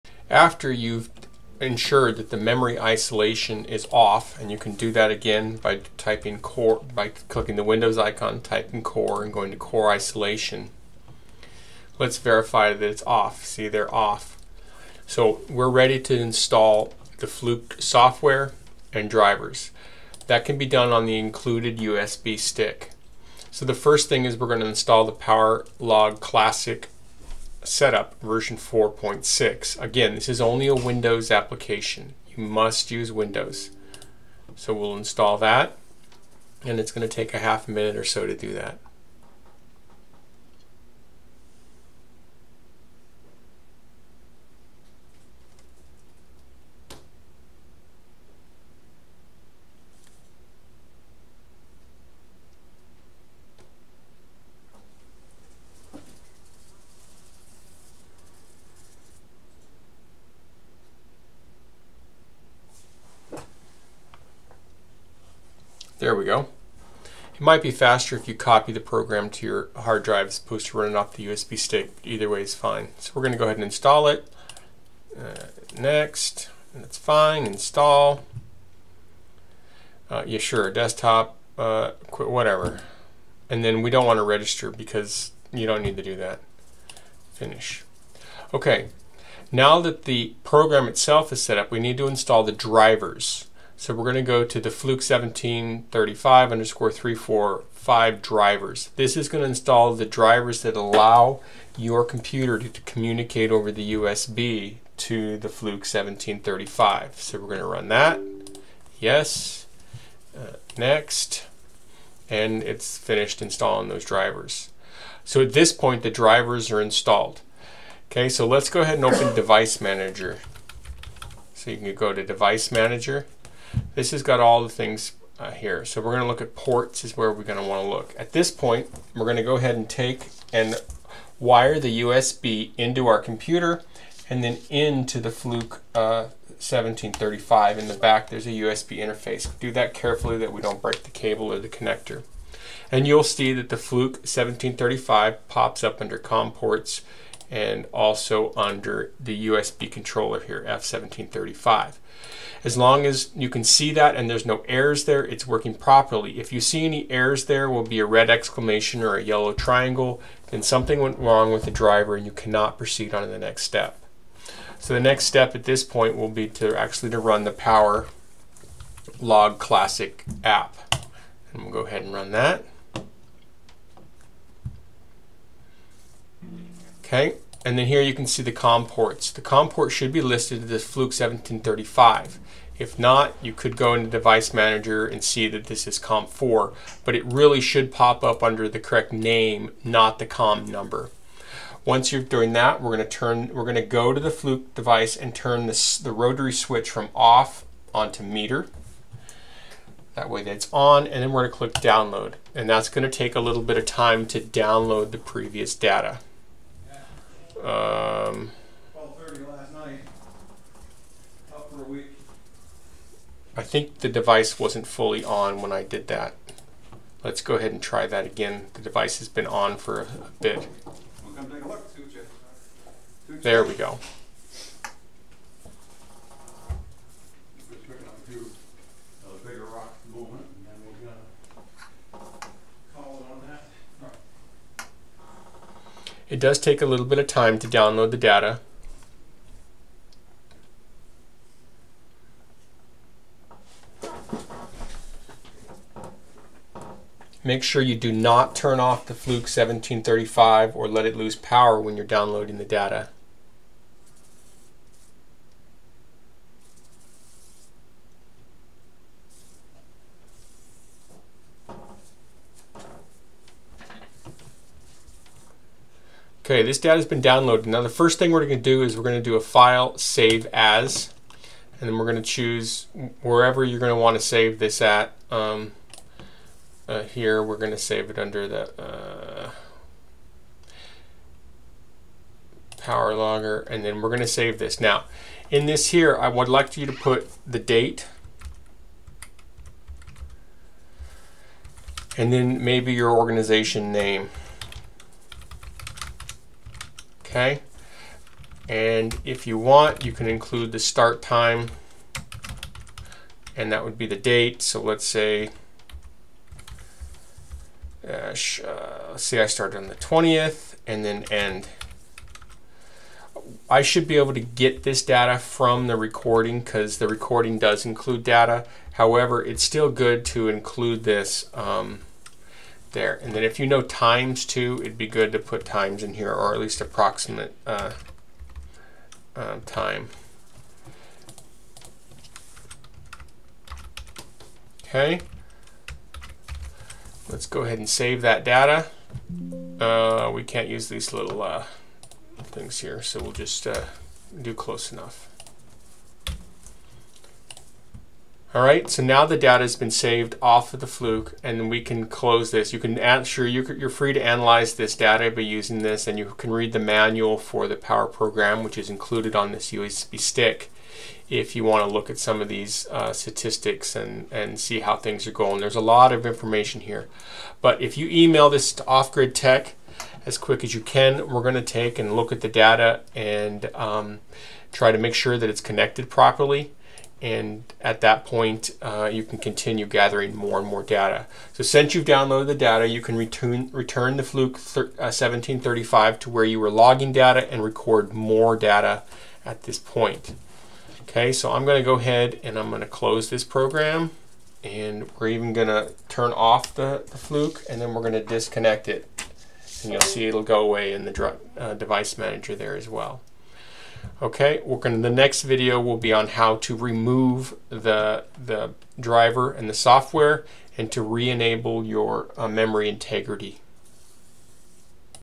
Video 2 Fluke 1735 Install Power Log and Drivers
Fluke 1735  Install Power Log and Drivers on Windows 10 or 11.

*Update: If you don't have the "Fluke1735_345_drivers.exe" file you can still manually install the drivers from the "Fluke 1735 345 drivers (.zip)" by extracting it. Then navigate to \F345_1735_WHQL_USB_driver\ and right click on the "ftdibus.inf" and  "ftdiport.inf" file and chose Install. Memory Integrity must be disabled.